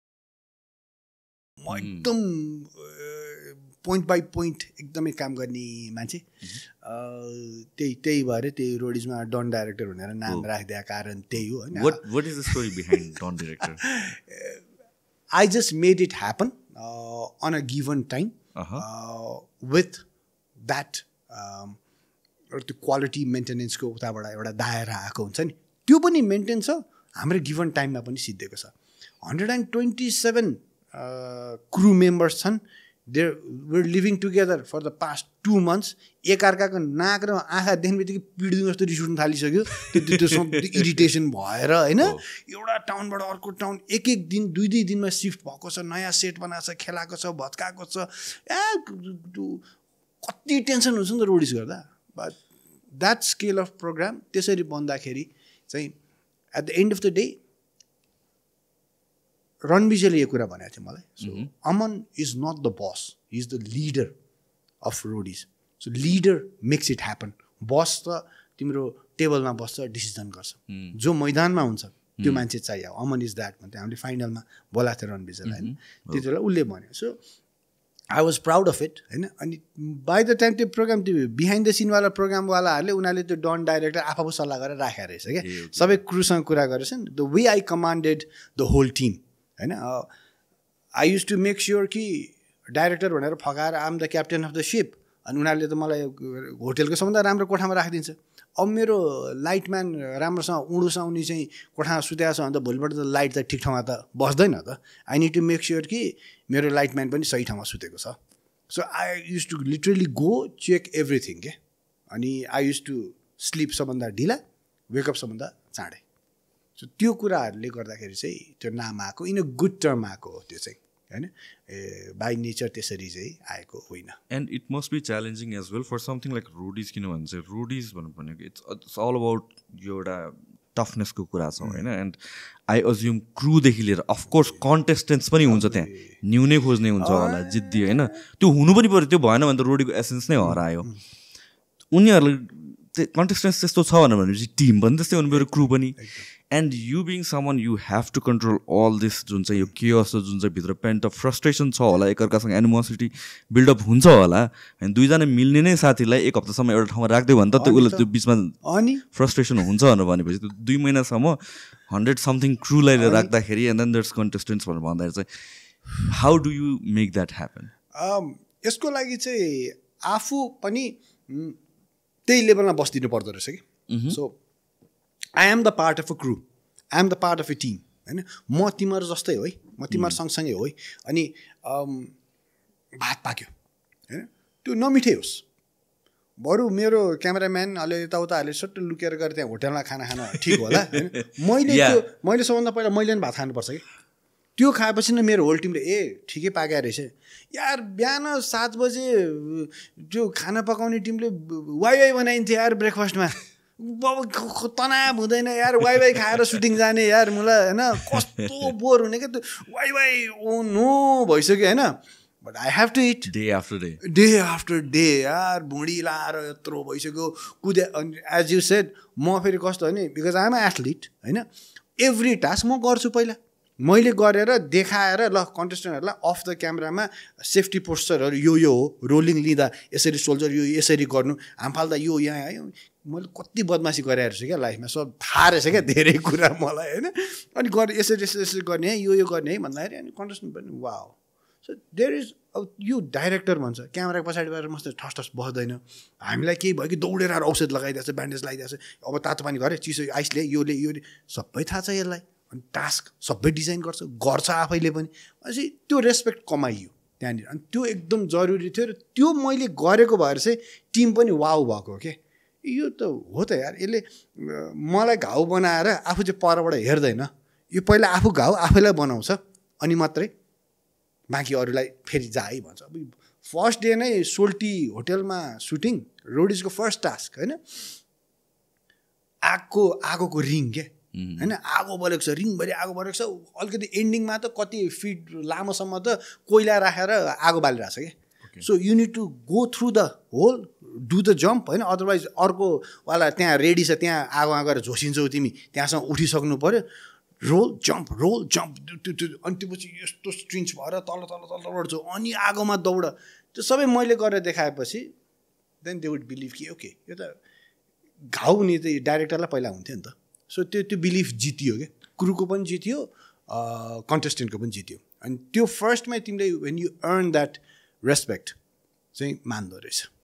Why Was Aman Pratap Adhikari Named 'Don Director'? | Sushant Pradhan Podcast
licensed professional specific to your needs.

Aman Pratap Adhikary is a versatile sports program presenter, producer, and director. In this insightful podcast where he shares his experiences in crafting shows like Himalaya Roadies, Ko Bancha Crorepati, and Poet Idol. From the early days of commentary to his recent return, Aman walks us down memory lane, offering a glimpse into the behind-the-scenes world of entertainment and sports broadcasting.

TEAM MEMBERS:
- HOST: Sushant Pradhan
- PRODUCER: Pratik Thapa